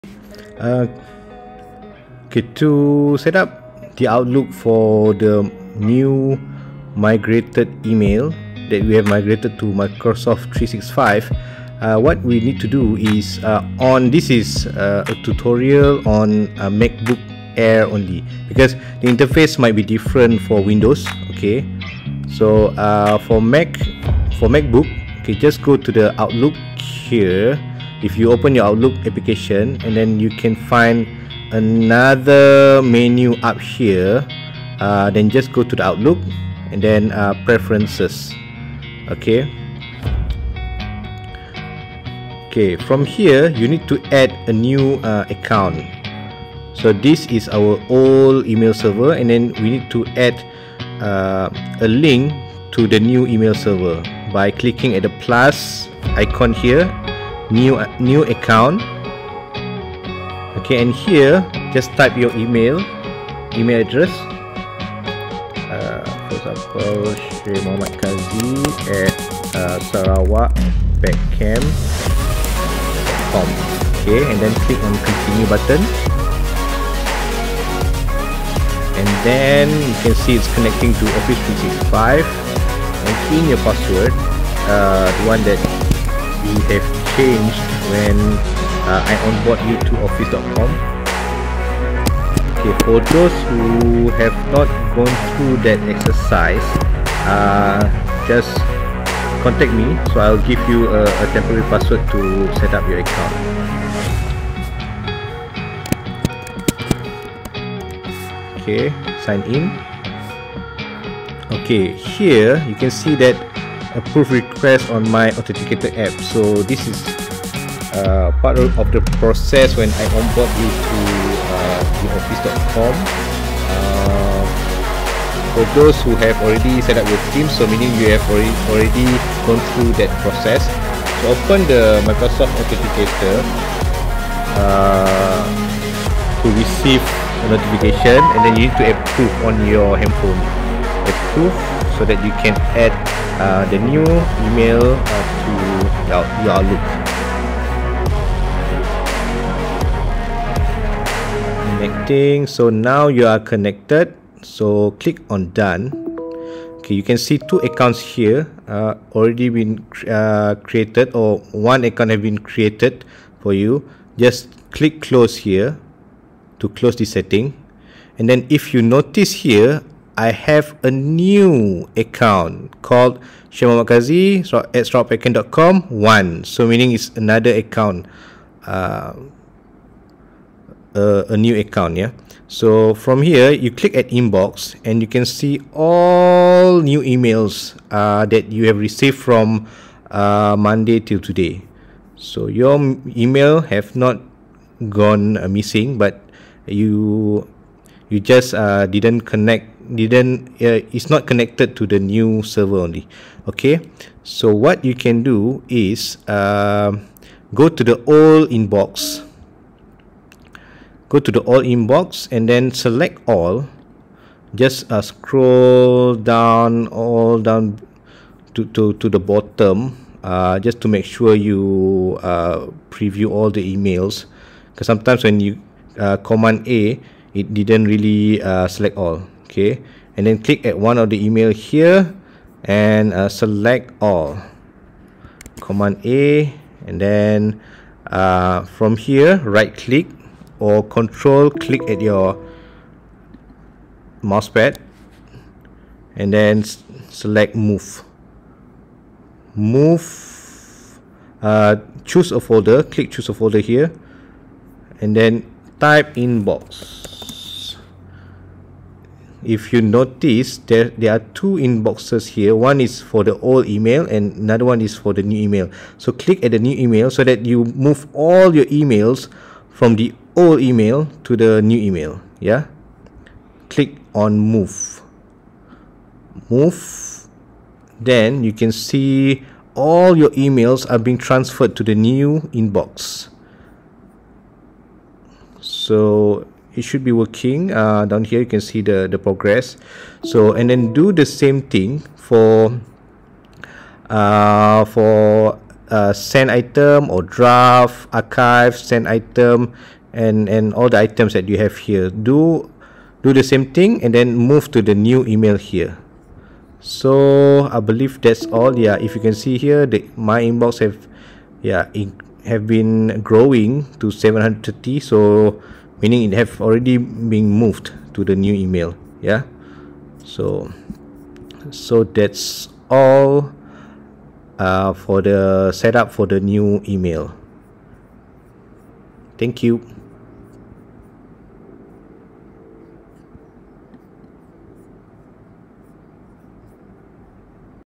How to setup new Microsoft 365 email on Macbook
Setting up Microsoft 365 email on Macbook Air.